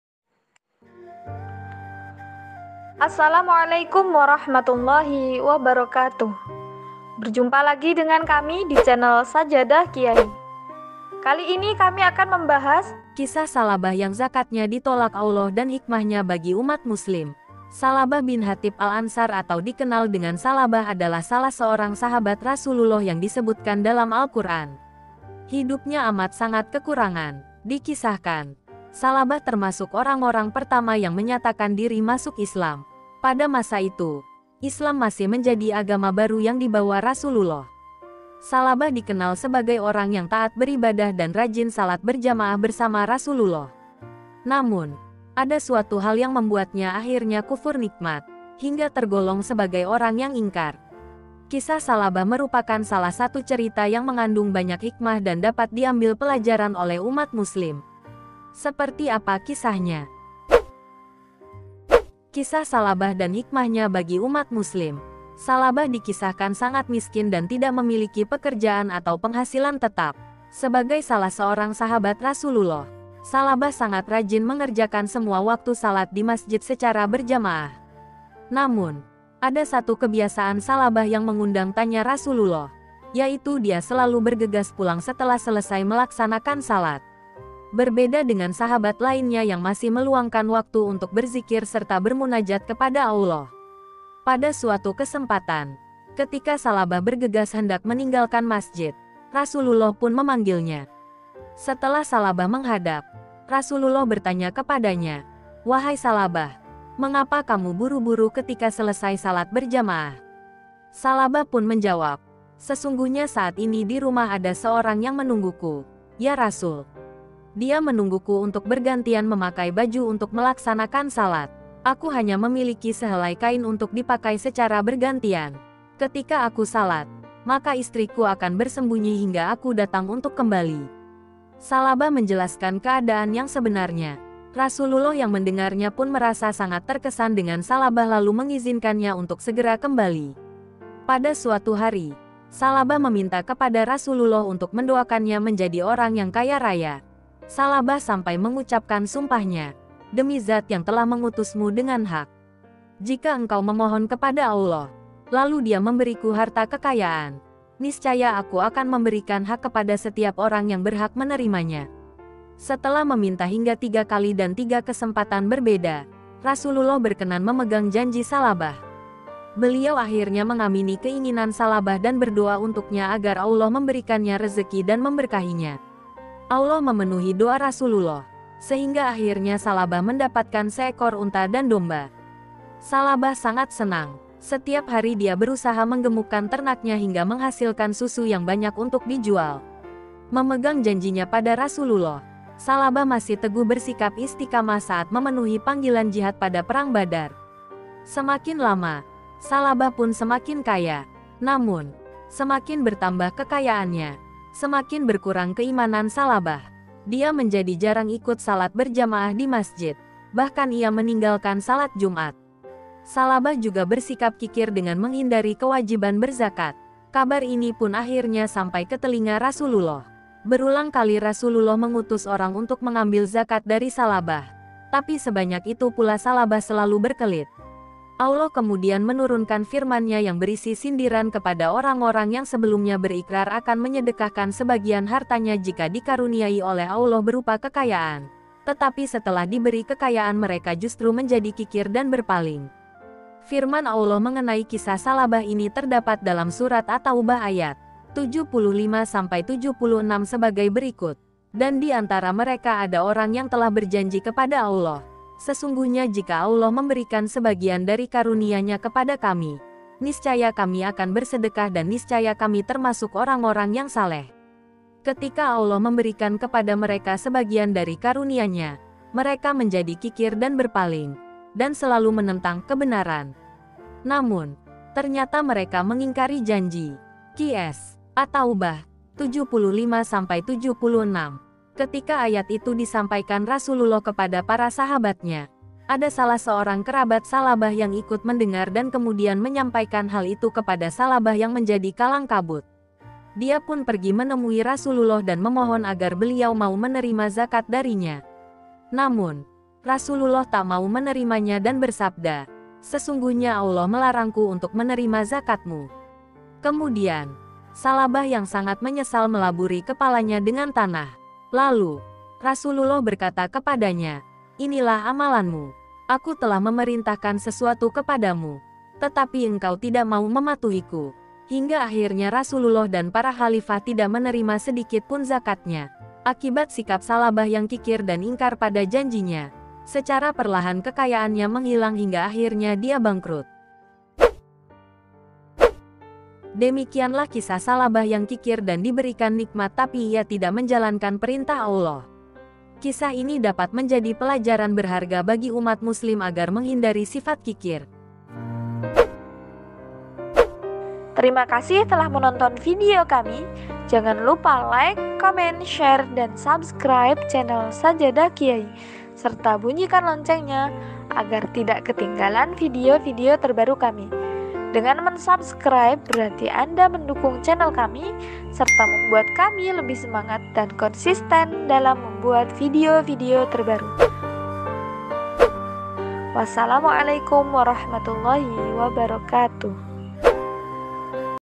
Zakatnya Di TOLAK ALLAH SWT⁉️Berikut Kisah TSA'LABAH BIN HAATHIB‼️
Kisah Tsa'labah yang Zakatnya Ditolak Allah dan Hikmahnya bagi Umat Muslim.

Tsa'labah bin Haathib Al-Anshar atau dikenal dengan Tsa'labah adalah salah seorang sahabat Rasulullah yang disebutkan dalam Alquran. Hidupnya amat sangat kekurangan.

Dikisahkan, Tsa'labah termasuk orang-orang pertama yang menyatakan diri masuk Islam. Pada masa itu, Islam masih menjadi agama baru yang dibawa Rasulullah.

Tsa'labah dikenal sebagai orang yang taat beribadah dan rajin salat berjamaah bersama Rasulullah. Namun, ada suatu hal yang membuatnya akhirnya kufur nikmat, hingga tergolong sebagai orang yang ingkar